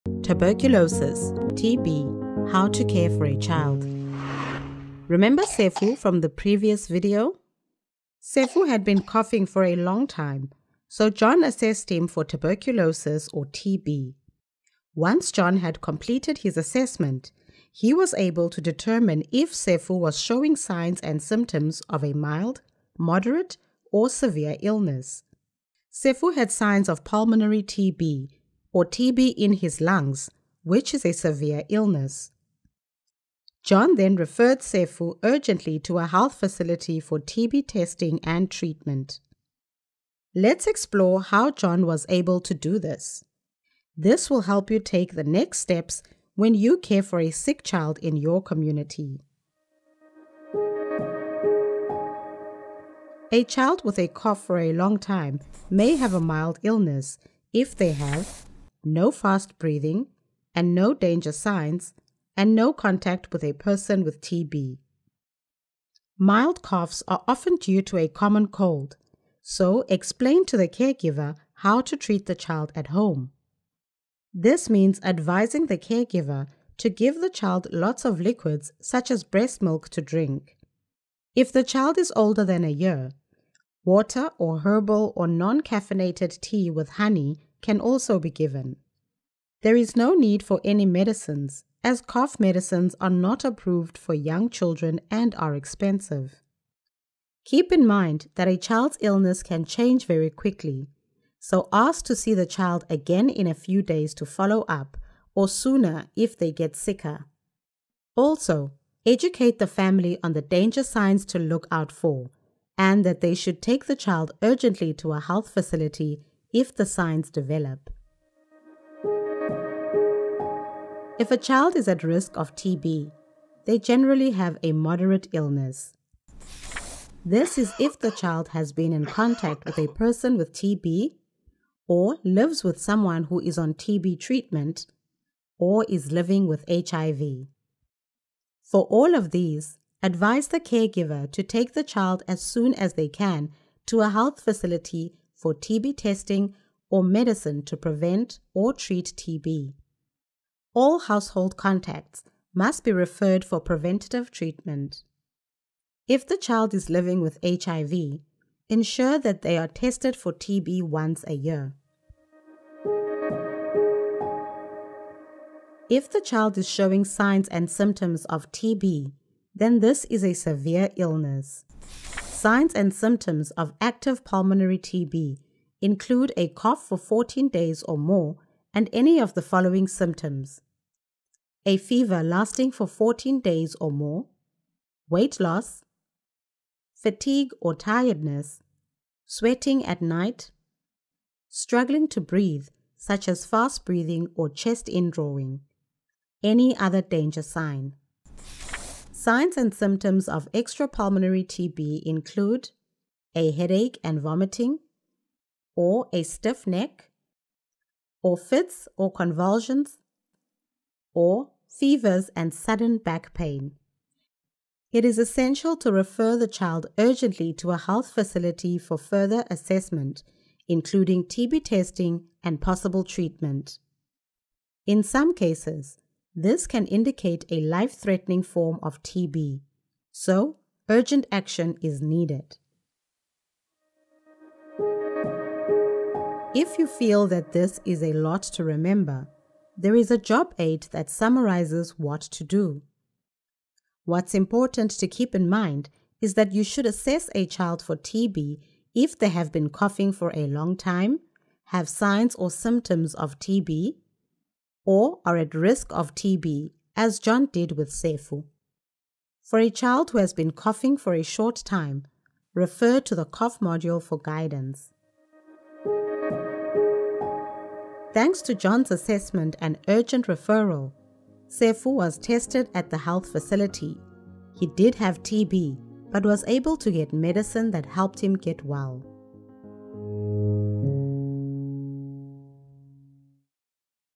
Tuberculosis: How to care for a child with TB
Once you have spoken to the child’s caregiver and examined the child for TB, you can determine how severe the child’s signs and symptoms are. This video reviews what to do next to care for a child who is between 2 months and 5 years of age.